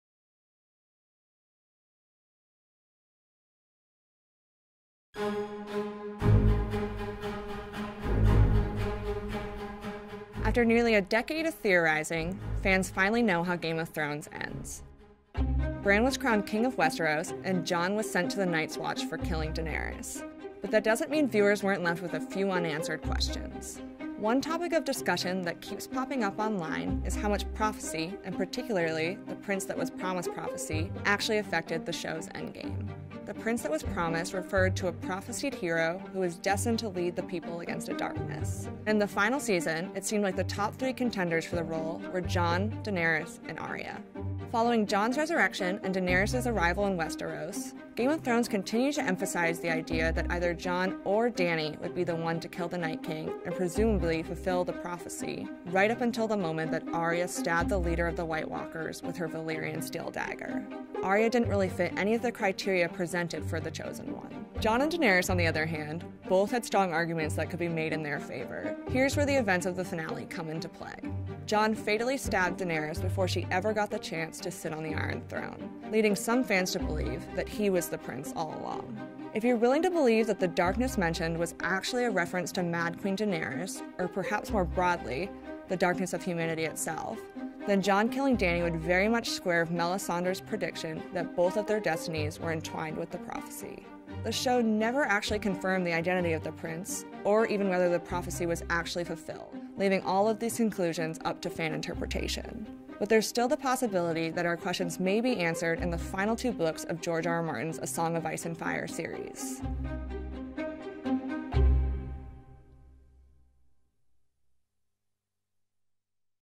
This Theory Was Left Unanswered In The Game Of Thrones Series Finale | TIME
If season 8 of Game of Thrones has taught us anything, it’s that prophecies aren’t always what they seem.